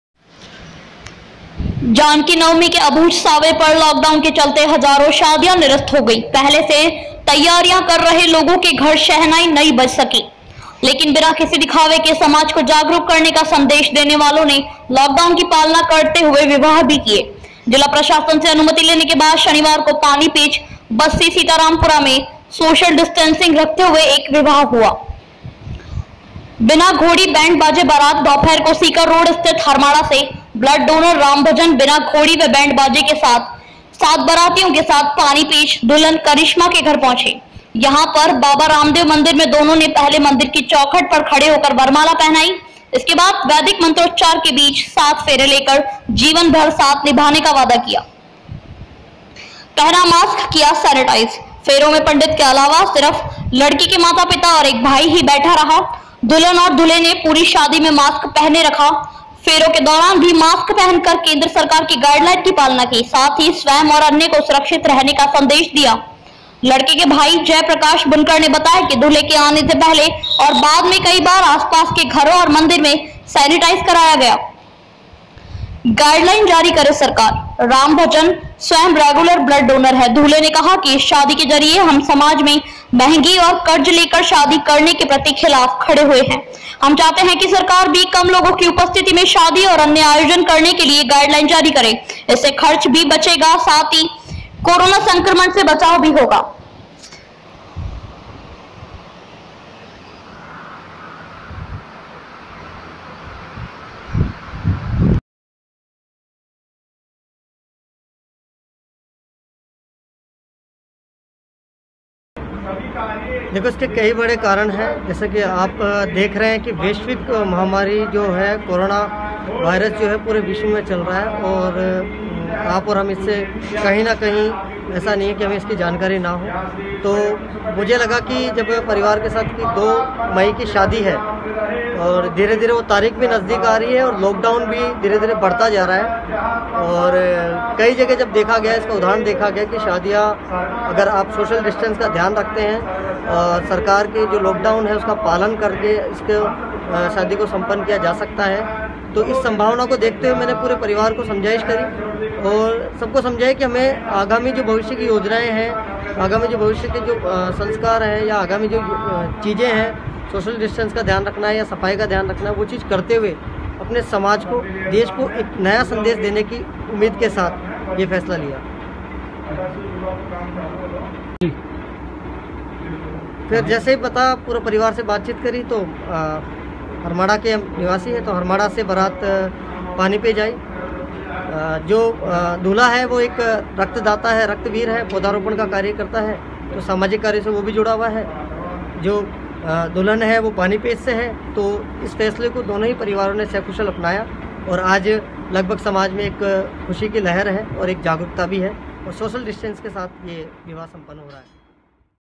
Lockdown में Marriage करना वाकई हिम्मत का काम
शादियों तो हर साल होती है, लेकिन Lockdown  में Marriage करना वाकई हिम्मत का काम है। Panipech BassiSitarampura में  2 मई Janki Navami को हुई। यहां पर Harmada से दुल्हे रामभजन बारात लेकर पहुंचे। बारात में 7 सात लोग ही थे। लडकी का नाम Karishma वर्मा है। यह शादी लडकी के घर के सामने स्थित Baba Ramdev Mandir में हुई।